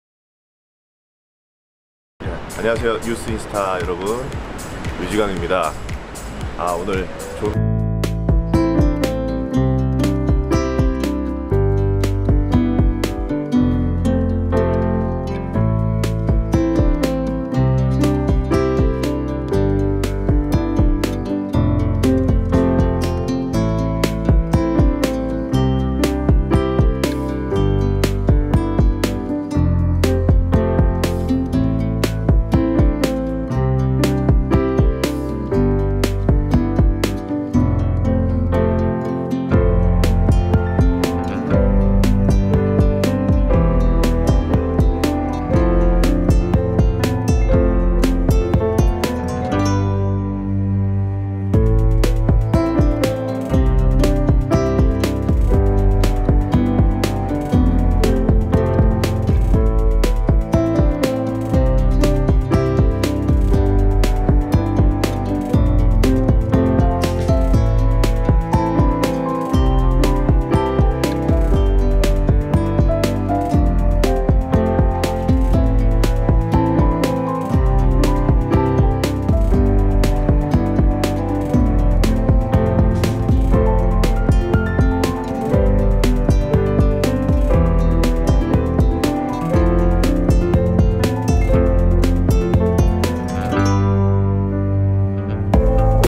동굴저음 류지광, 인간 버터 목소리로 전하는 인사 들어보셨나요? (KBS '가요무대' 출근길)
[뉴스인스타 양철수기자]   7월 6일(월) 오후 서울시 영등포구 여의도 KBS별관에서 진행되는 KBS 1TV ‘가요무대’ 출연을 위해 미스터트롯 류지광이 방송국으로 들어서고 있다.

류지광, 미스터트롯, 가요무대, 동굴 저음, 인간 버터, 출근길